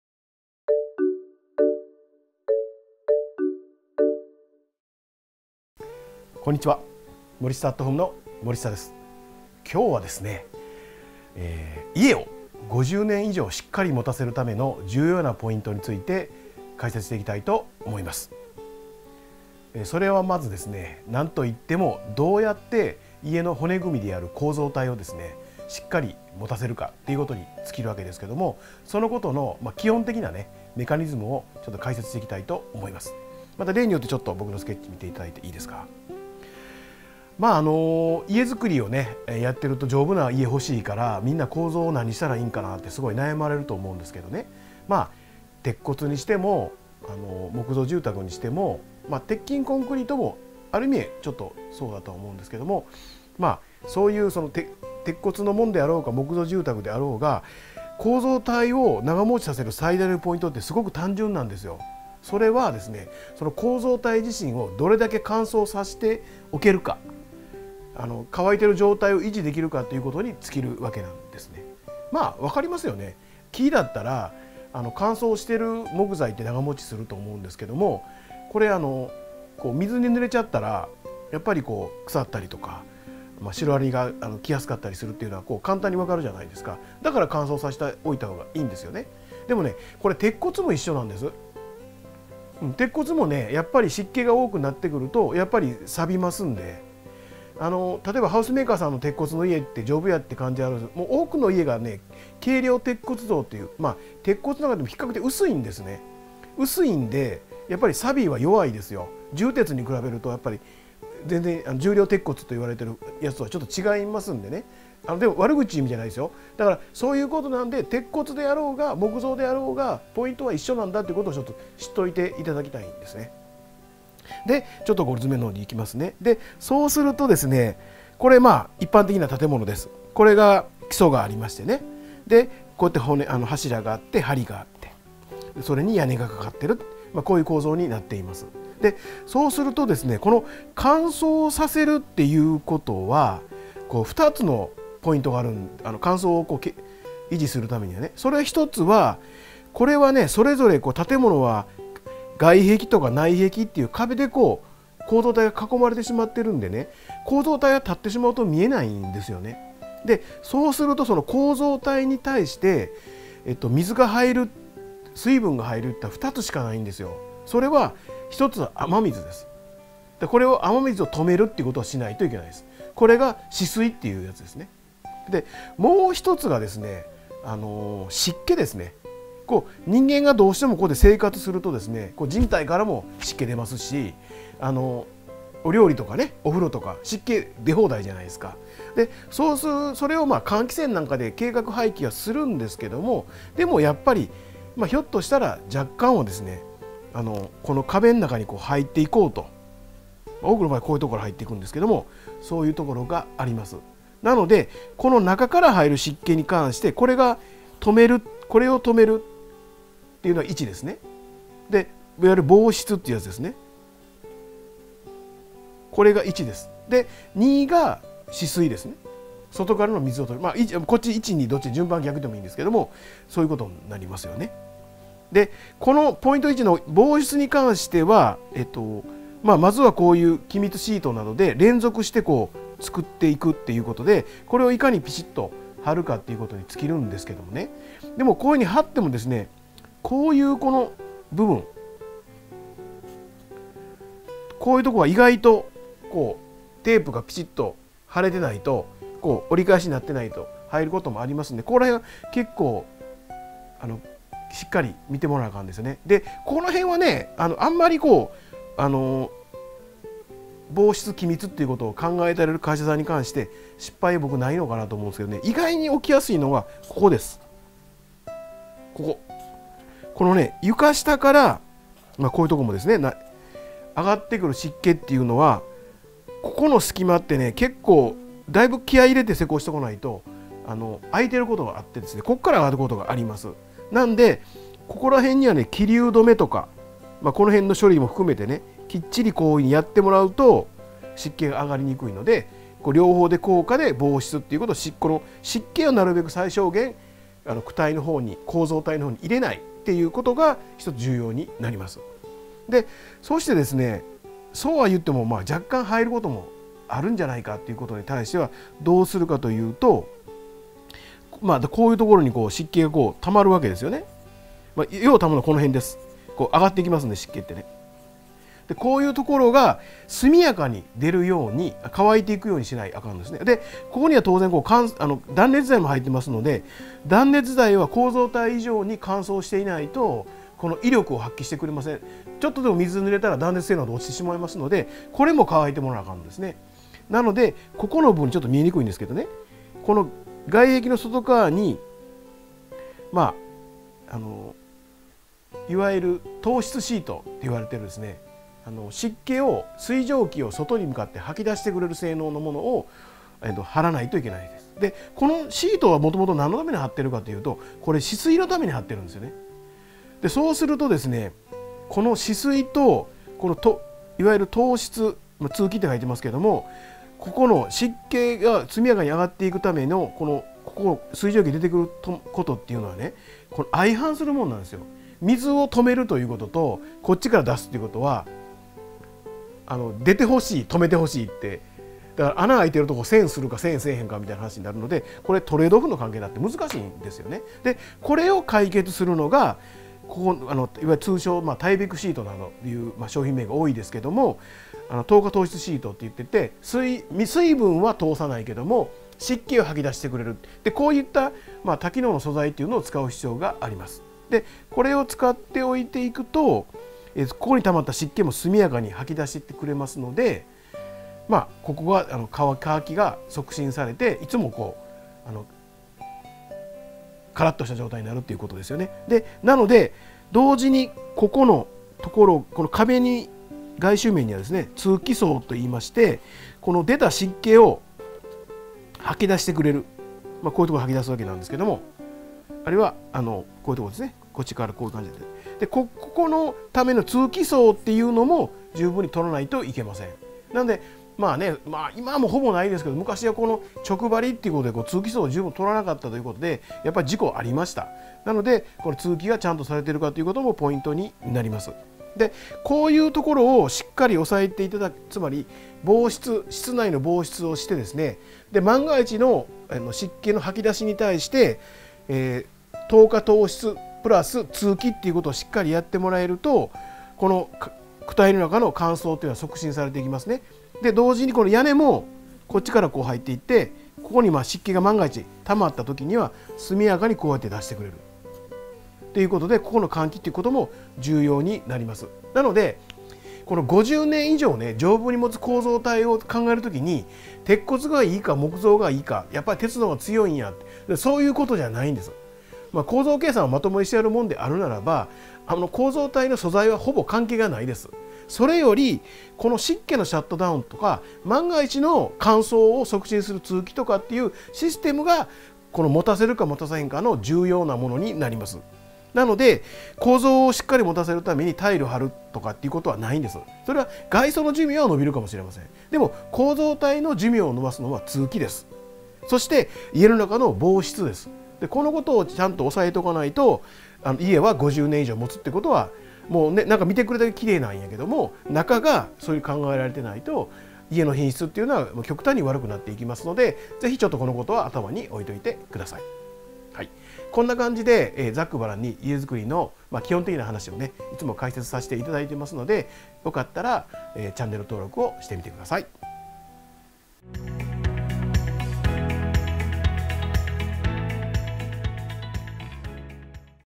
50年以上持つ家とは？（構造編）
今回は
家を５０年以上
持たせるための
重要なポイントについて
解説をします。

それは
家の骨組みである
構造体をしっかり
持たせるということに
尽きるわけです。

基本的なメカニズムを
解説します。

家づくりは
みなさん丈夫な
家が欲しいから
どんな構造が良いか
すごく悩まれると
思います。

鉄骨や木造住宅、
鉄筋コンクリートも
そうだと思います。

これがなにであれ、
構造体を長持ちさせる
ポイントはすごく
単純です。

それは構造体自身を
どれだけ乾いている
状態に維持できるか
ということに
尽きるわけです。

乾燥している木材は
長持ちすると
思いますが、
水に濡れると
腐ったりシロアリが
来やすかったりするのは
簡単にわかりますよね。

だから
乾燥させておいた方が
いいんですよね。

これは鉄骨も一緒です。

鉄骨も湿気が
多くなってくると
錆びます。

だから
鉄骨であろうが
木造であろうが
ポイントは一緒です。

この乾燥を維持する
というポイントが
２つあります。

1つ目は、
水分対策です。

構造体に対して
水分が入る可能性は
２つしかありません。

一つは雨水です。
止水ですね。

もう一つは
湿気ですね。

人間が室内で
生活をすると、
人体から湿気が
出ますし、
料理やお風呂からも
湿気が出ます。

それを換気扇等で
計画排気をしますが、
それでも若干は
壁の中に入って
いこうとします。

なので中から入る
湿気を止めることが
必要です。

いわゆる
防湿と言われる
ものです。

この防湿に関しては
気密シートを
連続して貼るので、
いかにピシッと
貼るかが大事です。

意外と気密テープが
貼れてないとか、
シートが折り返しに
なっていないとか
ありますのでしっかり
見てもらわないと
いけません。

床下から上がってくる
湿気はだいぶ気合いを
入れて施工をしないと、
湿気が上がることが
あります。

なので気流止めなど
きっちりやってもらうと
湿気が上がりにくいので
防湿をして湿気を
なるべく構造体の方に
入れないということが
重要になります。

そうは言っても
若干入ることも
あるので、その場合は
速やかに出るように
乾いていくように
しないといけません。

しかも
壁には断熱材も
入っています。

断熱材は
構造体以上に
乾燥していないと
威力を発揮して
くれません。

断熱材が少しでも
水に濡れたら
断熱性能は
落ちてしまいます。

なので外壁の外側には
いわゆる透湿シートと
言われている、湿気を
外に出してくれる
ものを貼らないと
いけません。

このシートは
もともとは止水のために
貼っています。

そうすると、
止水と透湿通気は、
書いてますけど
相反するものです。

これを解決するのが
通称タイベックシート
という商品名が多いですが
透過透湿シートと言って
水分は通さないけど
湿気をは出してくれる
という多機能の素材を
使う必要があります。

これを使うと、
溜まった湿気も
速やかに出してくれるので
乾きが促進されて
いつもカラッとした
状態になるということです。

今はほぼ無いですが、
昔は直張りで通気層を
十分に取らなかった
ことで事故がありました。

なので通気が
ちゃんとされてるか
ということも
ポイントになります。

室内の防湿をして、
万が一のときは
湿気を出してくれる。

透過透湿＋通気を
しっかりやると、
躯体の中の乾燥が
促進されていきます。

なので50年以上、
丈夫に持つ構造体を
考えるときは、
鉄骨か木造かという
ことではありません。

構造計算をまともに
していれば、
構造体の素材は
ほぼ関係がないです。

それよりも、
湿気のシャットダウンや
万が一のときの乾燥を
促進する通気を持たせるか
持たさないかが
重要になります。

これらを押さえて
おかないと、
家の品質は極端に
悪くなっていくので、
頭に置いておいて
ください。